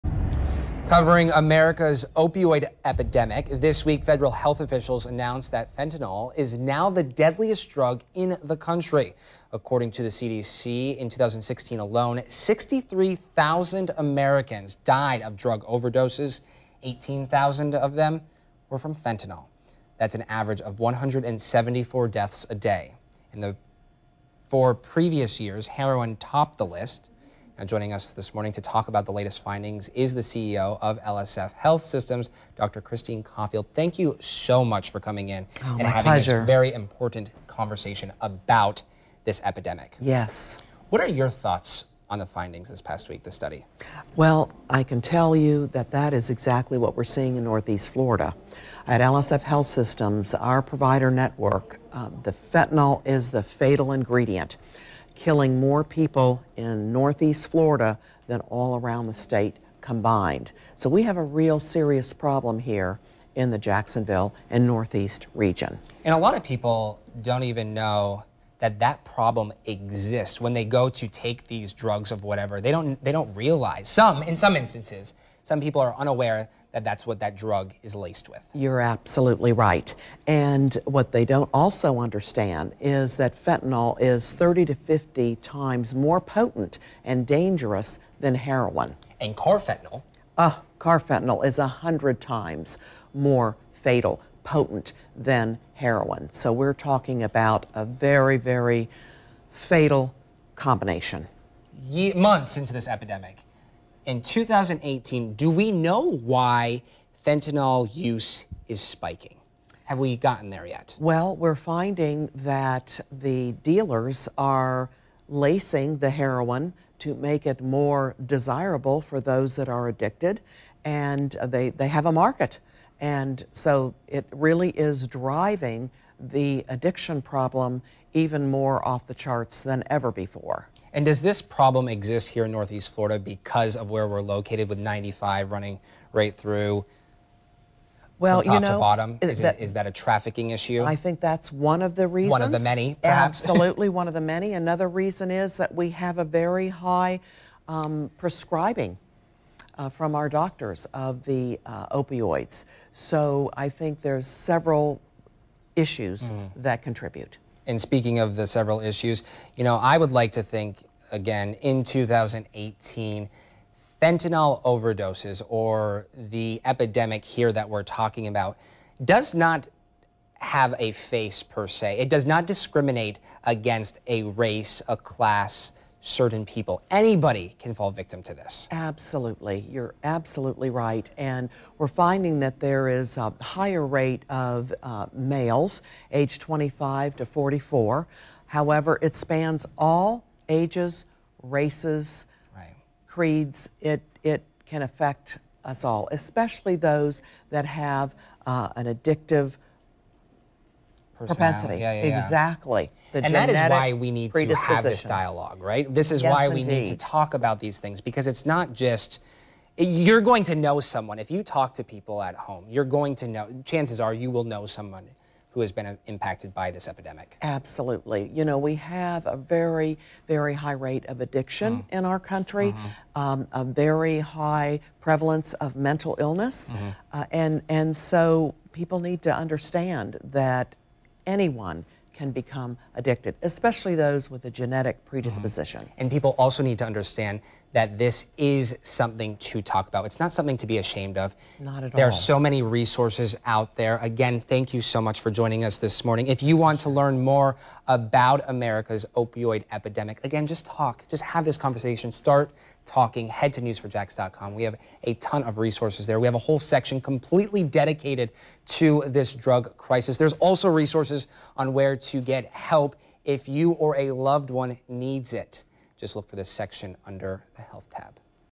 Fentanyl now the most dangerous drug
VIDEO: Zac Lashway talks with Dr. Cauffield about opioids.